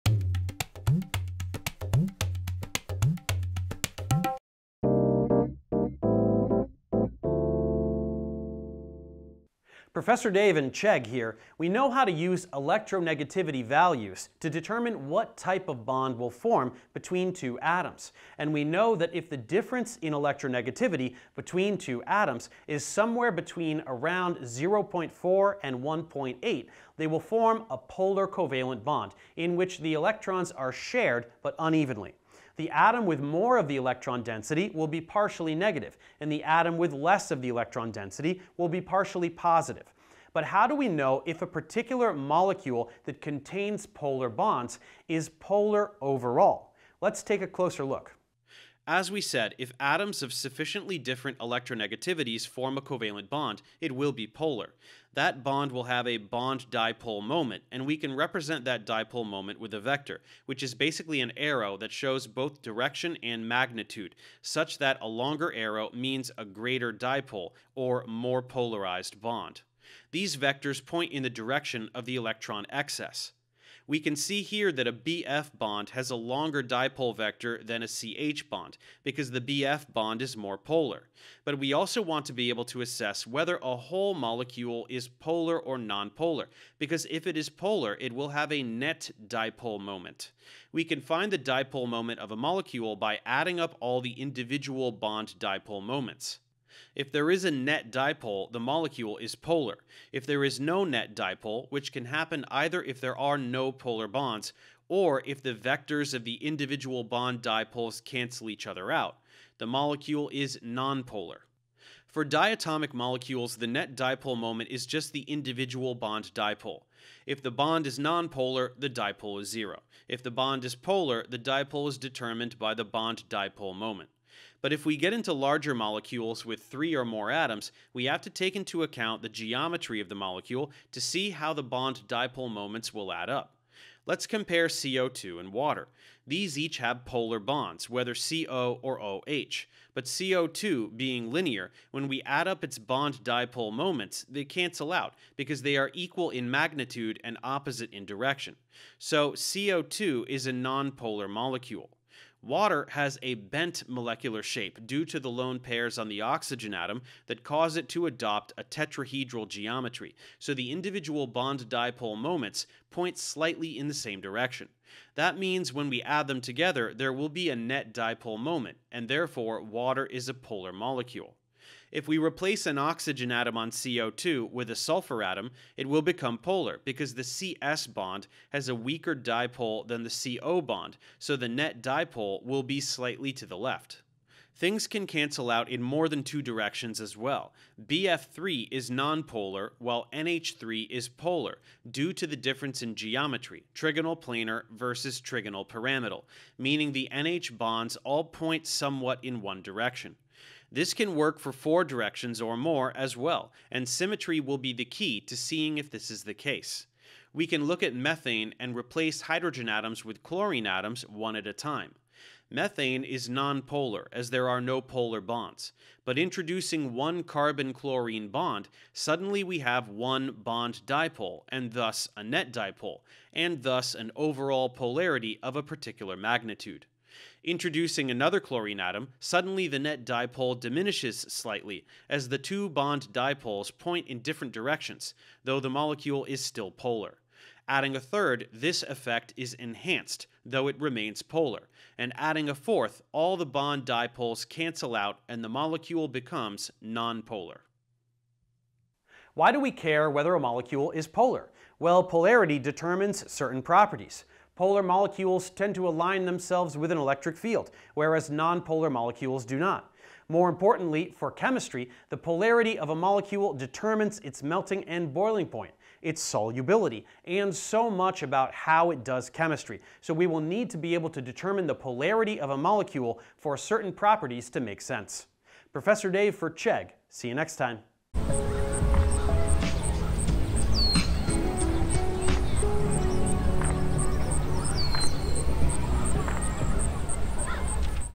Bond Polarity and Dipole Moments | Professor Dave & Chegg Explain
We know that if the difference in electronegativity between two atoms is somewhere between 0.4 and 1.8, they will form a polar covalent bond. How do we know if a particular molecule that contains polar bonds is polar overall? Let's take a closer look at polar covalent bonds, bond dipole moments, net dipole moments, and the process for assessing the polarity of a molecule. We'll also learn how geometry becomes a factor in larger molecules.

00:00 Intro
00:48 Bond dipoles
01:14 Net dipole moments
01:30 Adding bond dipole vectors